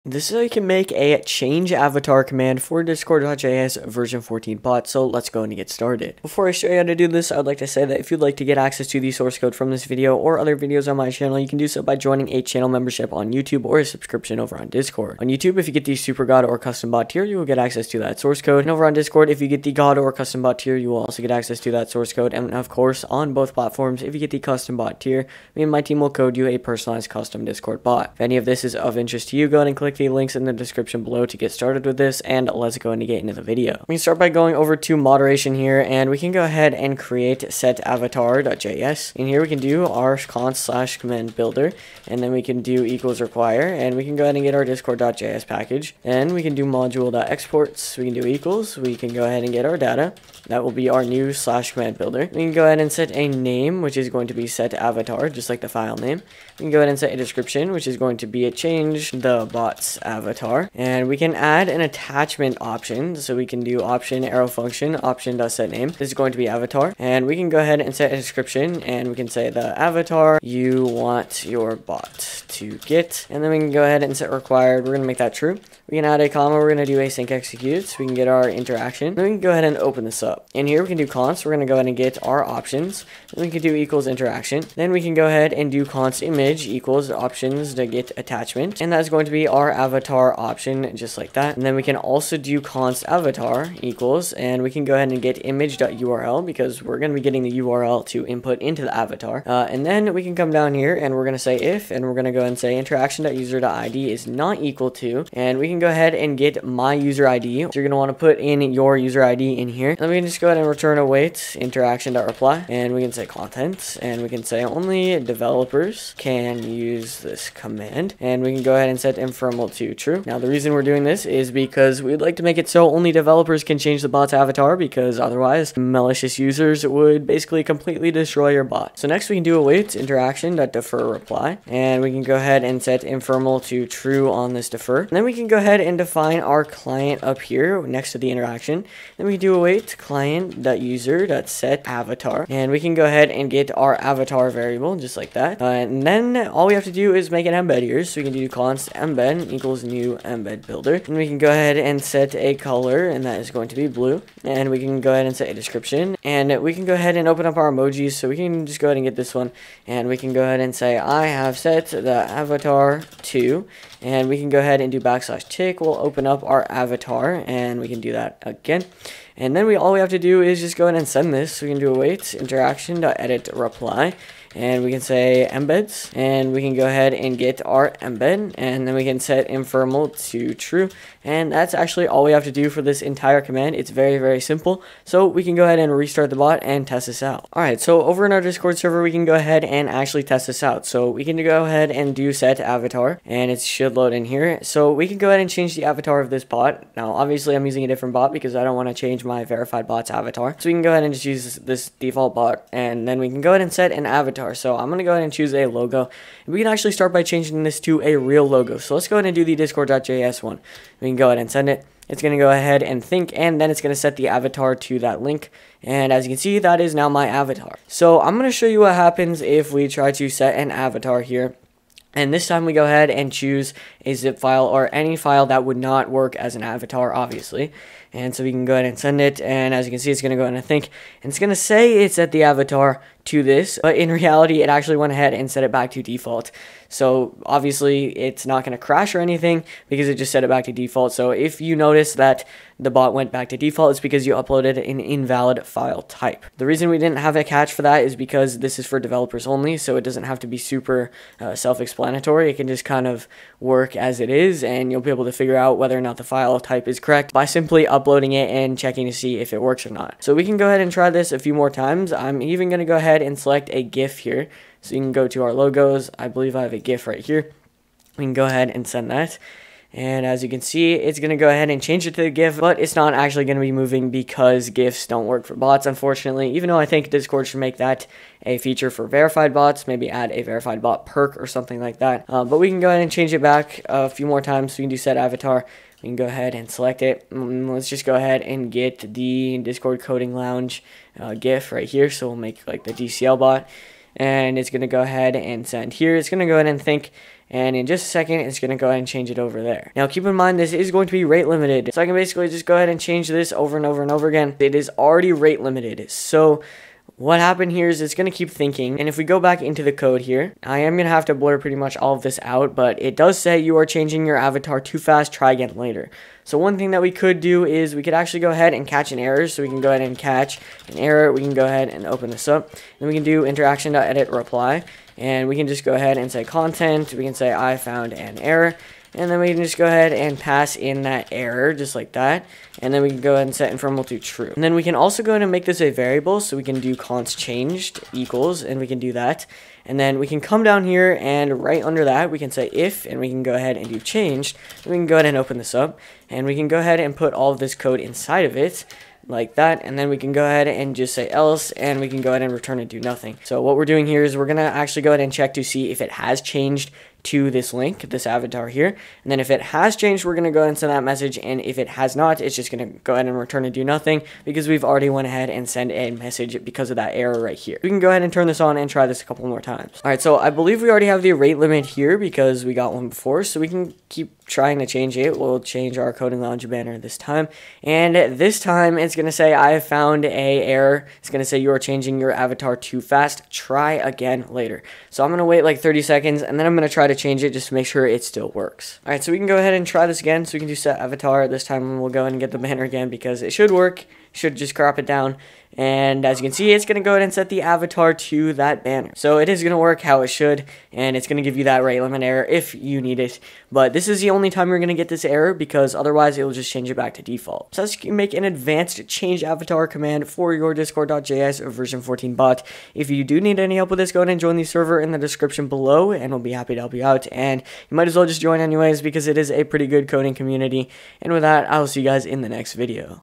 [NEW] - How to make a CHANGE AVATAR COMMAND for your discord bot! || Discord.js V14
This is how you can change your discord bot's avatar with a command!